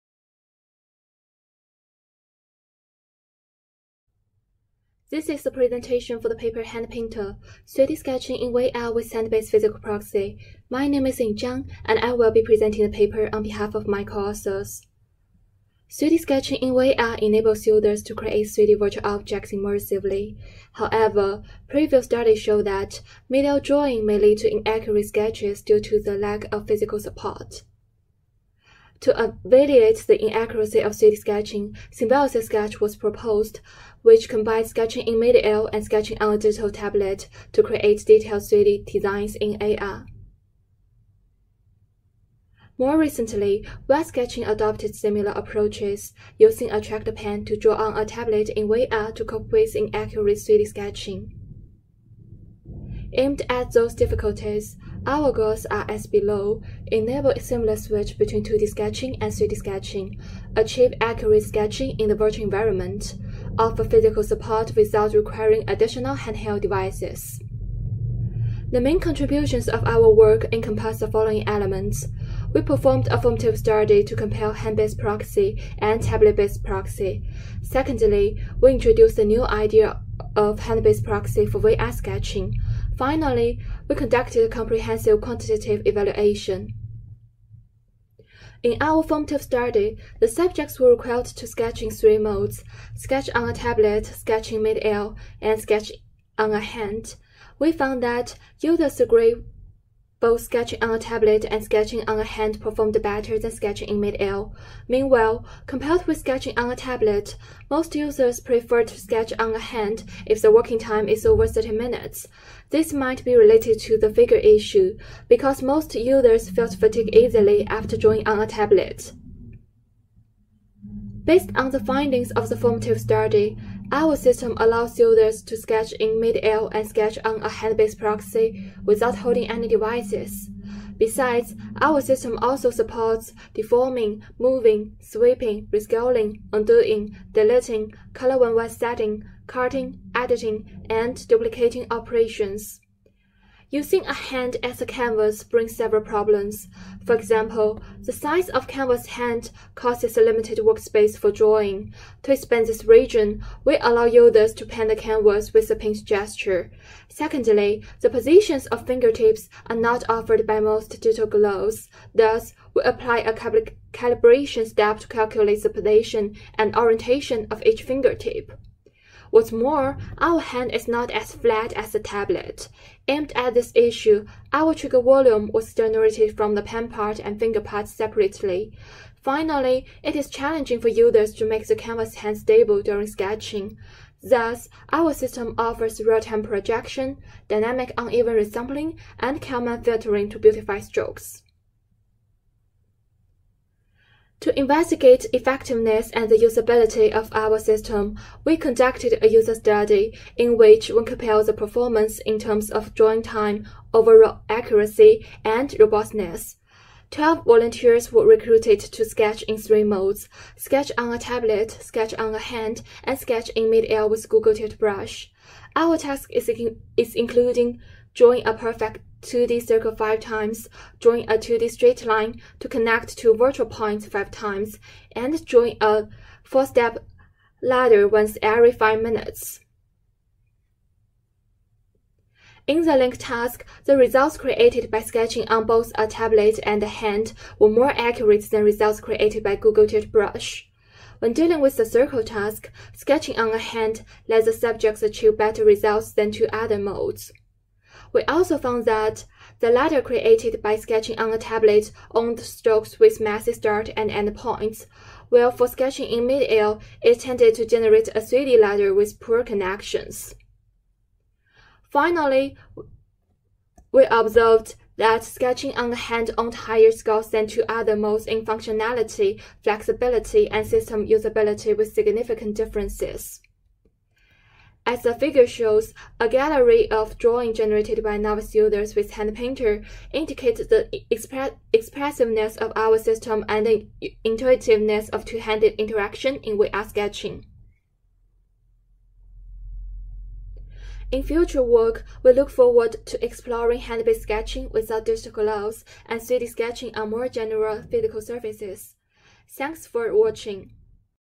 HandPainter --- 3D Sketching in VR with Hand-based Physical Proxy
Ying Jiang, Congyi Zhang, Hongbo Fu, Alberto Cannavò, Fabrizio Lamberti, Henry Y K Lau, Wenping Wang

Session: Engineering Real-World Interaction

Abstract
3D sketching in virtual reality (VR) enables users to create 3D virtual objects intuitively and immersively. However,  previous studies showed that mid-air drawing may lead to inaccurate sketches. To address this issue, we propose to use one hand as a canvas proxy and the index finger of the other hand as a 3D pen. To this end, we first perform a formative study to compare two-handed interaction with tablet-pen interaction for VR sketching. Based on the findings of this study, we design HandPainter, a VR sketching system which focuses on the direct use of two hands for 3D sketching without requesting any tablet, pen, or VR controller. Our implementation is based on a pair of VR gloves, which provide hand tracking and gesture capture. We devise a set of intuitive gestures to control various functionalities required during 3D sketching, such as canvas panning and drawing positioning. We show the effectiveness of HandPainter by presenting a number of sketching results and discussing the outcomes of a user study-based comparison with mid-air drawing and tablet-based sketching tools.